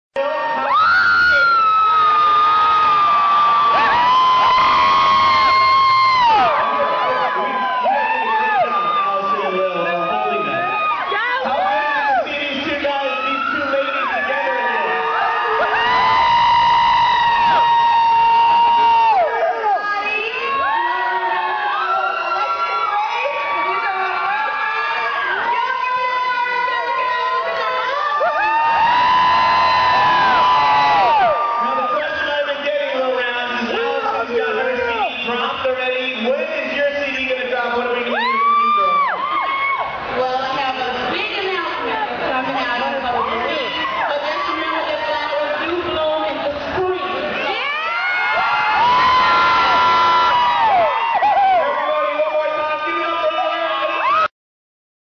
Allison Iraheta and Lil Rounds - Jingle Jam 2009
Allison Iraheta and Lil Rounds presenting a check at the Q107.5 Jingle Jam.
Thanks so much to Janfan211 ;] she's awesome for letting us use this!